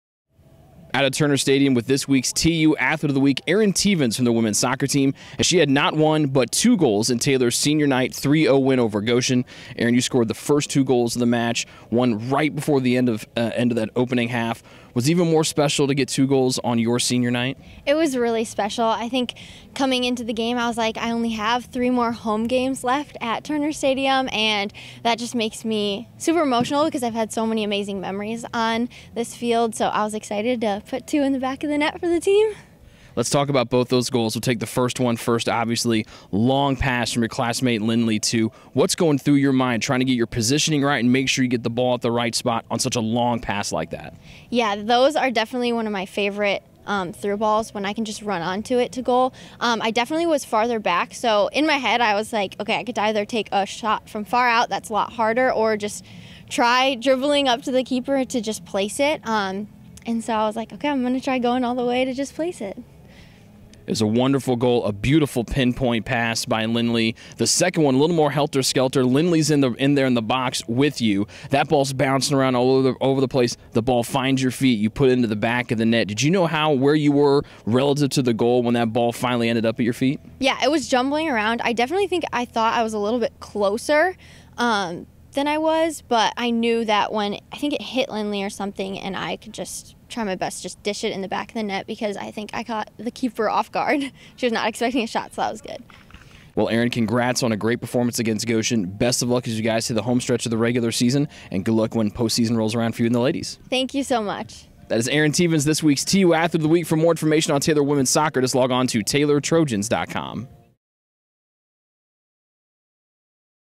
TU Athlete of the Week | Erin Teevans
This week's TU Athlete of the Week is forward Erin Teevans! The senior scored twice in TU's 3-0 win over Goshen last Saturday.